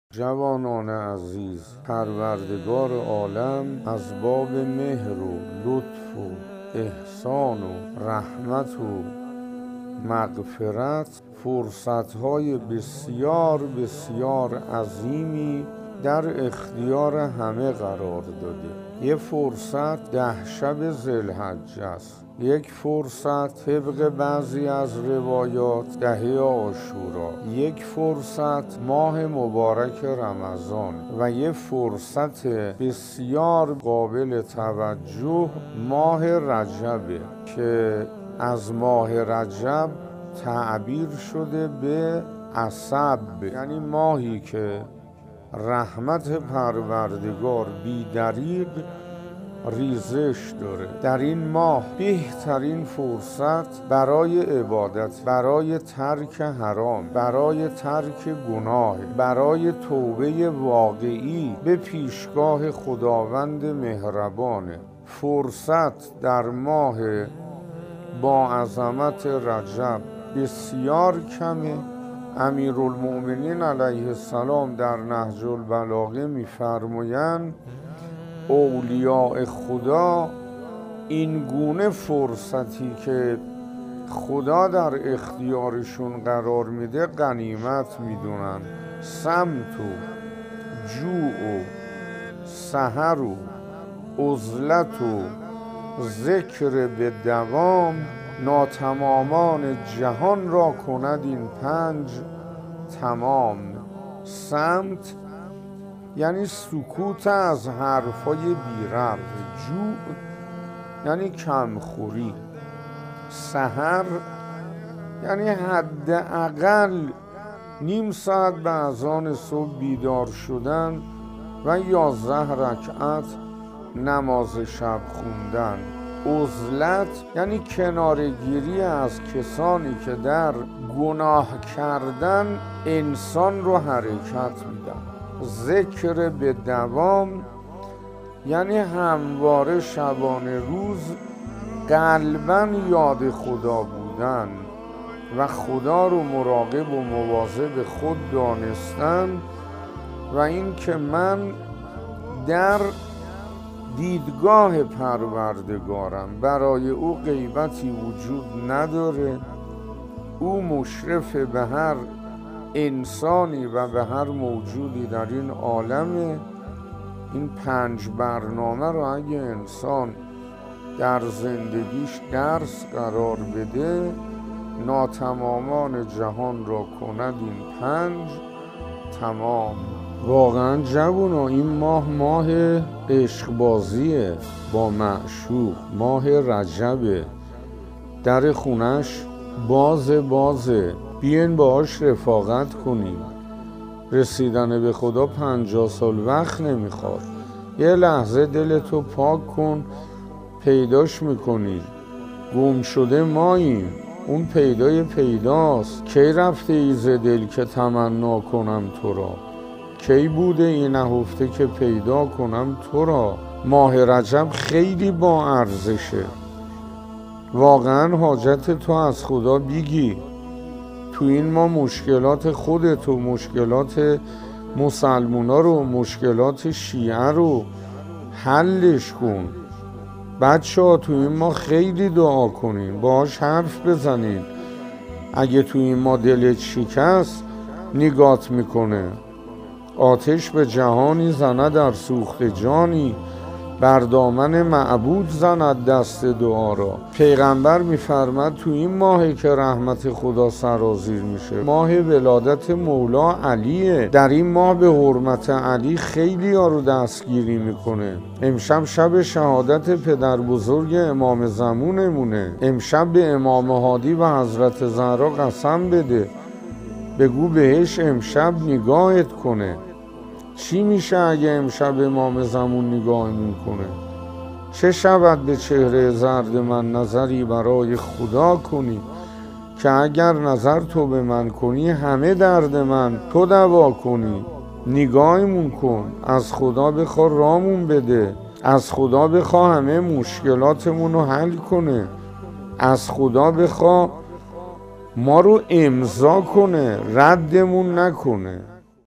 توی این ماه چه خبره ؟ - شیخ حسین انصاریان
کلیپ توی این ماه چه خبره ؟ / حجم 40 مگابایت/ زمان 5 دقیقه / توصیه های استاد درباره ماه رجب

ما را در اینستاگرام دنبال کنید